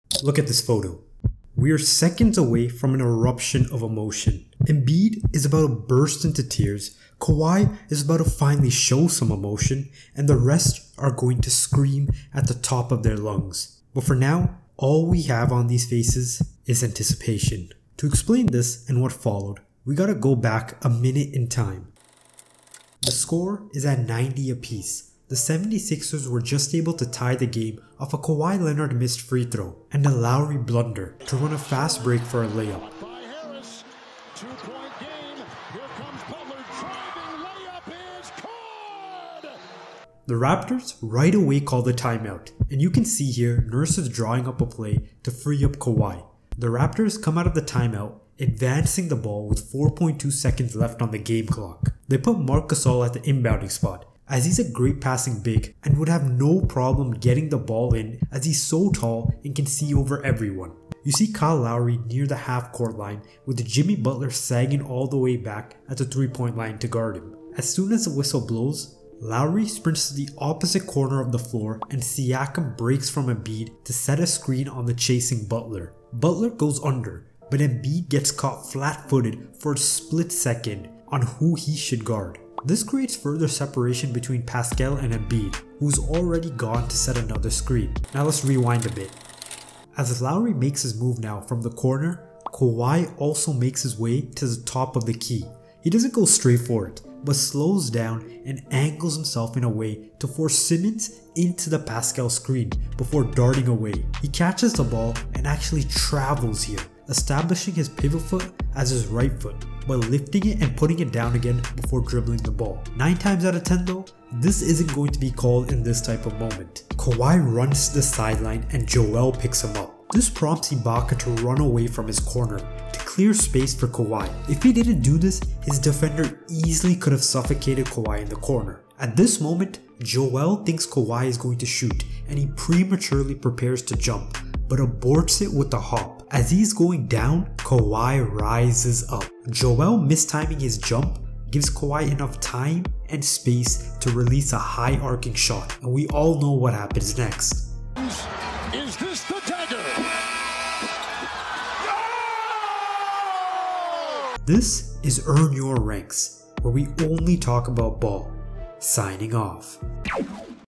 Breakdown of Kawhi Leonard's Series Clinching Game Winner
Kawhi Leonard hit one of the greatest shots in sports history. His game-winner was a movie ending. A game 7 buzzer-beater shot that had 4 bounces before it went in. It was spectacular to watch and one that will never be recreated again. It was a miracle game-winner and a series clinching shot. In this video, we will breakdown the play leading up to the greatest game-winner in basketball and sports history.